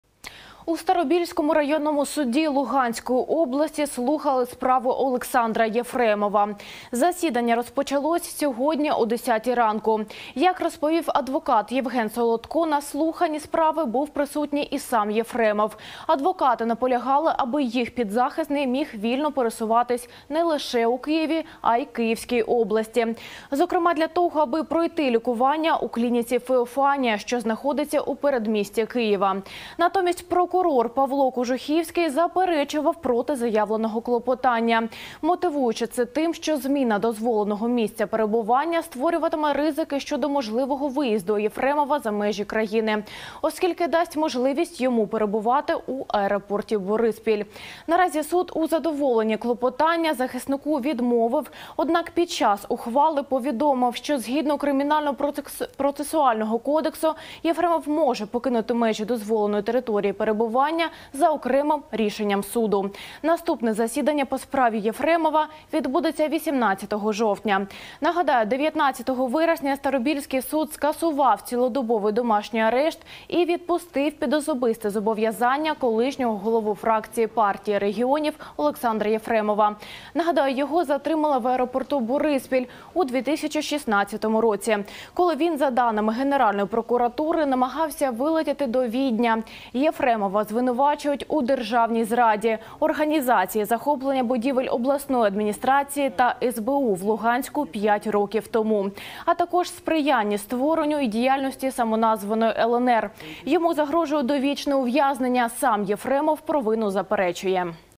У Старобільському районному суді розглядають справу Єфремова
У Старобільському районному суді Луганської області заслухали справу Олександра Єфремова. Засідання розпочалось сьогодні о 10 ранку. Як розповів адвокат Євген Солодко, на слуханні справи був присутній і сам Єфремов.